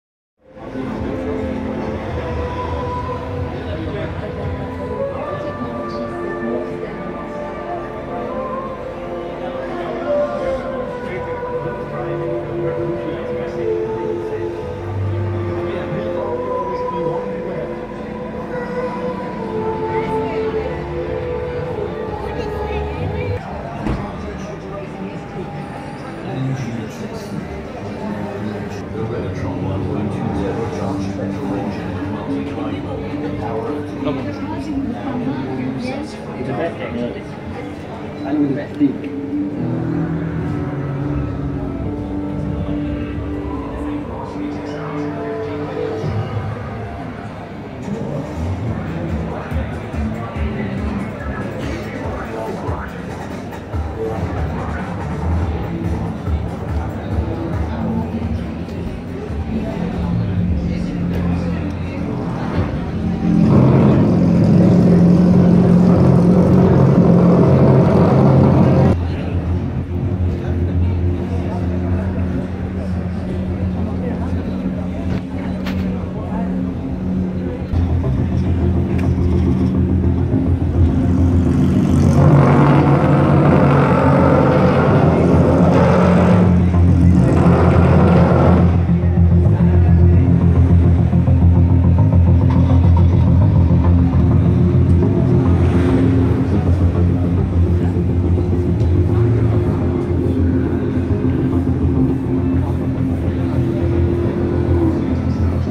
Mercedes Benz GLA and CLA Walkaround HD
Mercedes Benz GLA and CLA in Times Auto expo Chennai 2015